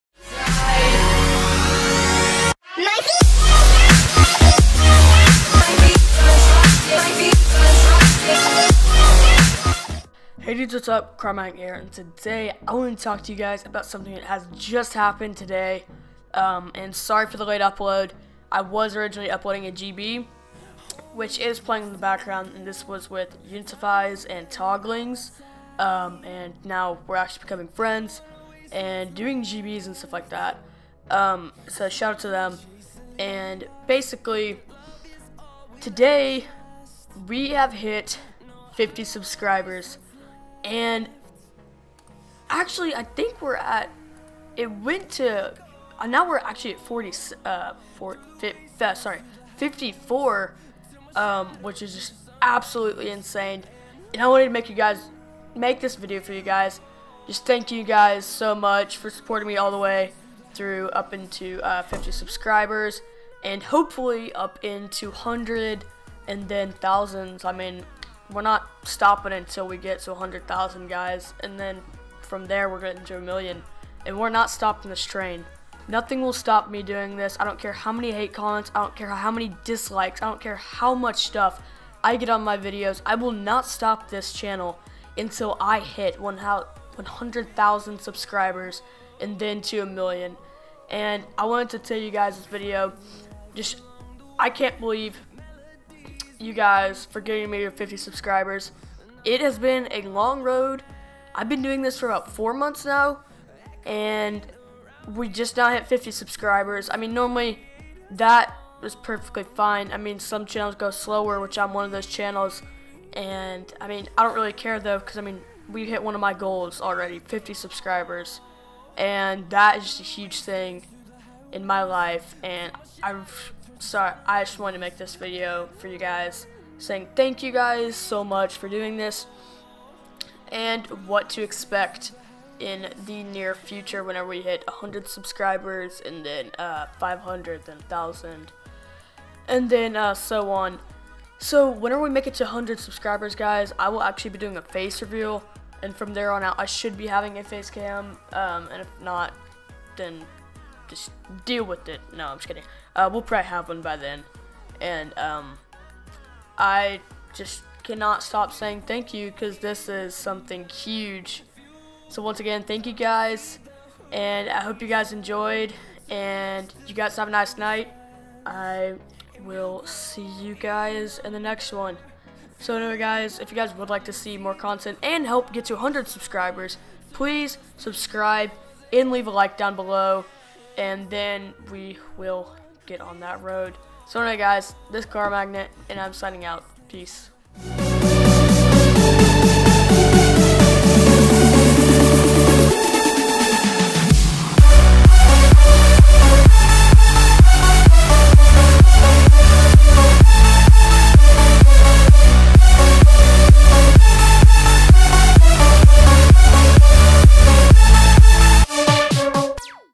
WE HIT 50 SUBS!!
THANK YOU ALL SOOO MUCH! I cannot even begin where to start! But, we are not stopping! Thank you all again so much!